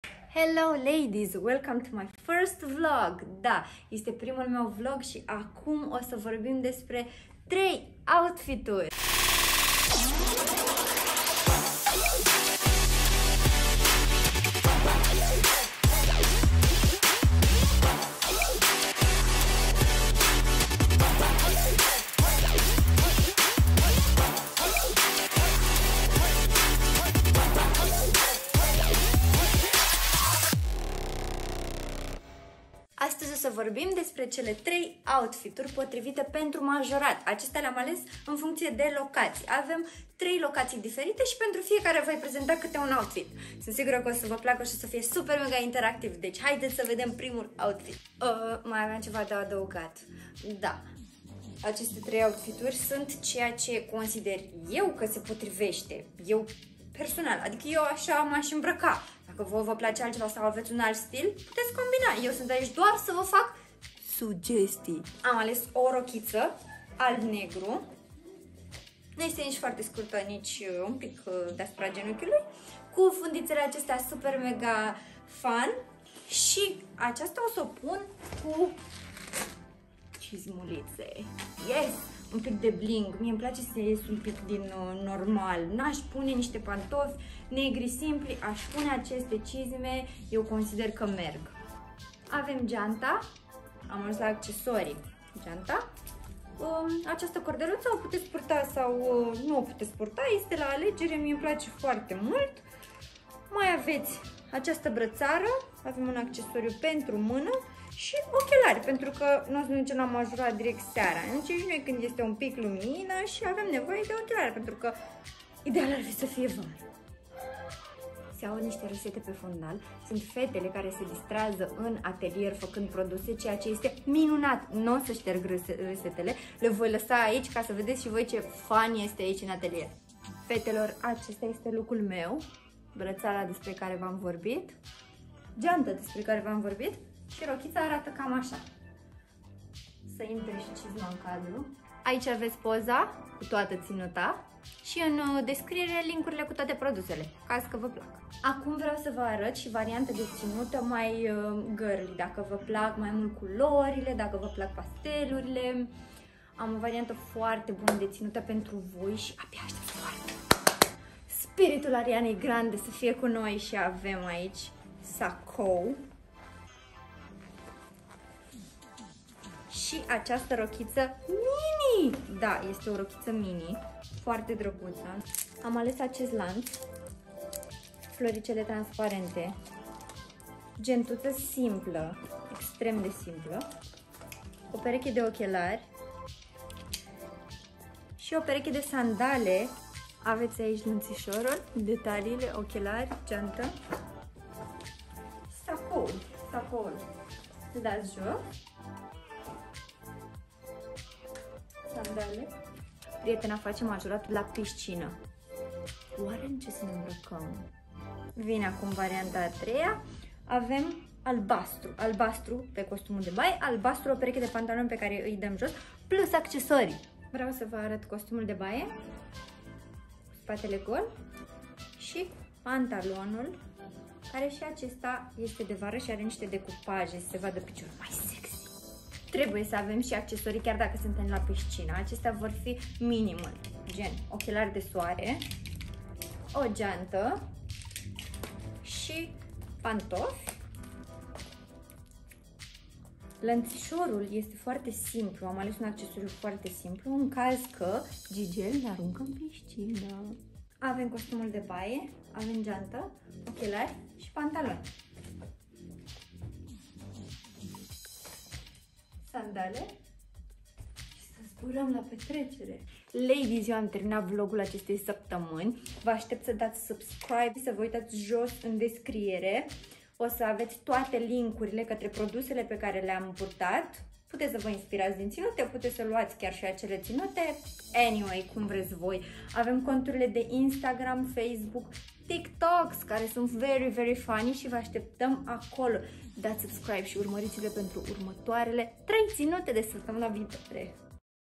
3 TINUTE PENTRU MAJORAT
HELLO LADIES ! I
Iselin ma numesc si designer vestimentar sunt.
Acesta este primul meu vlog cu sugestii. Aveti 3 variante de tinute pentru majorat. Hope you enjoy it! Aveti tag in descriere tuturor brandurilor de unde am achizitionat produsele si tineti minte Ladies: ”Eu sunt aici ca sa sugerez, nu ca sa dictez!”

OUTFIT NUMBER 1
Dress: ISELIN DIMITRI
Shoes:TEZYO
Bag: Guess
Accesories:H&M, Roberto Cavalli

OUTFIT NUMBER 2
Dress: ISELIN DIMITRI
Shoes: ZARA
Sunglasses, Necklace:H&M
Bag:COTTON

OUTFIT NUMBER 3
Pants&Bathingsuit: ISELIN DIMITRI
Bag: TEZYO
Shoes: ZARA
Sunglasses: H&M